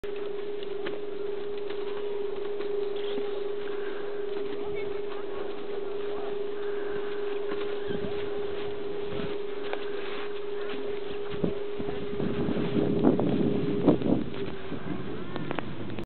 Daniel beim Snowboarden
Daniel beim Snowboarden in Nendaz, Wallis, Schweiz